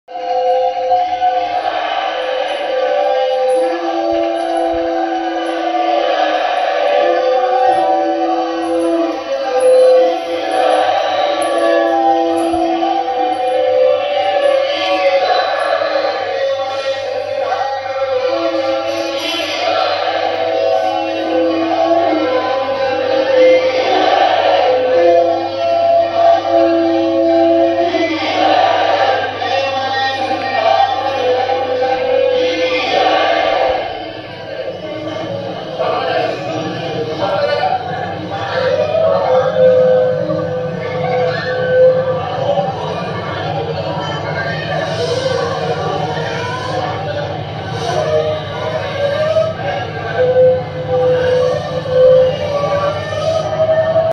Holy Inauguration of Satsang Vihar Ranchi by Rev Sipai Dada 💞
Holy Inauguration of Satsang Vihar Ranchi by Rev Sipai Dada on 10th March 2024 💞